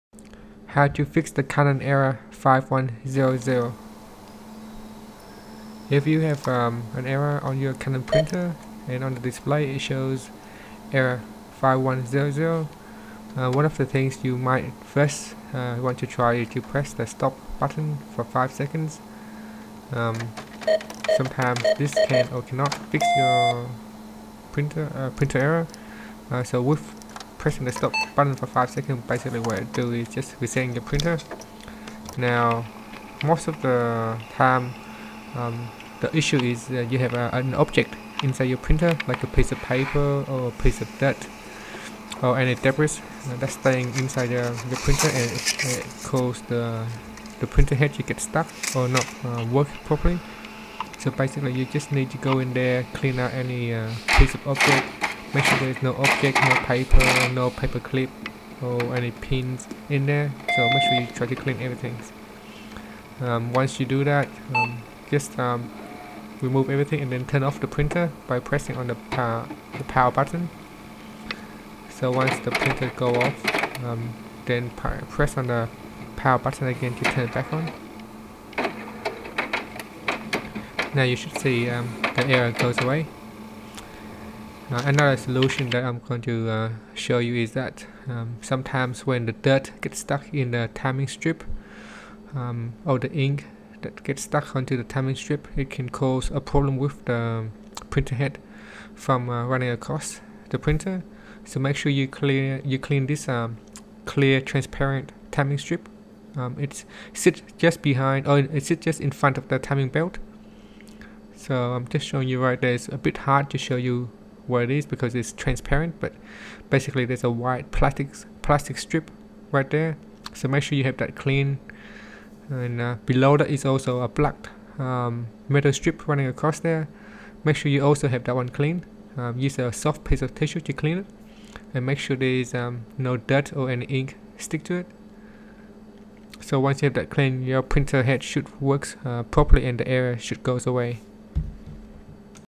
How To Fix Canon Printer Error 5100
Learn how to fix the Canon printer error 5100, and have your printer running again.